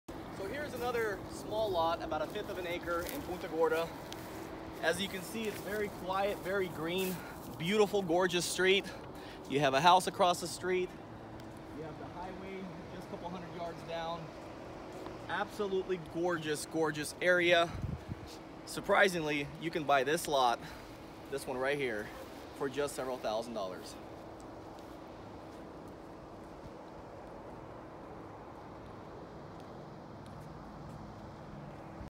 Owner Finance Property in Florida! Cheap Price!
Great Opportunity to own a beautiful lot in Punta Gorda, Florida with zero down! More details